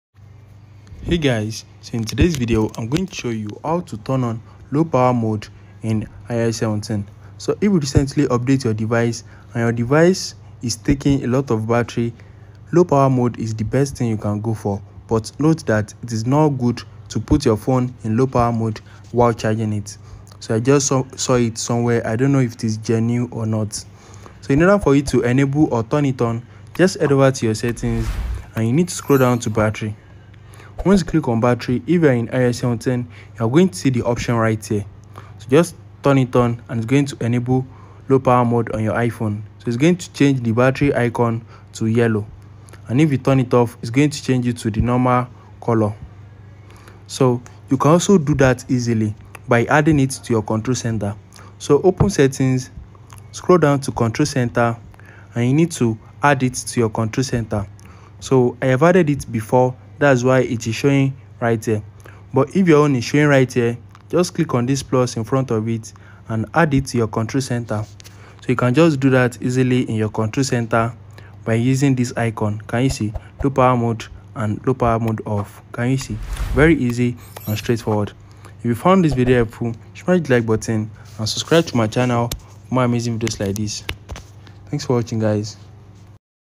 How to Turn On Low Power Mode on iPhone - iOS 17
Learn how to activate Low Power Mode on your iPhone with our simple guide. Conserve your battery and keep your device running longer!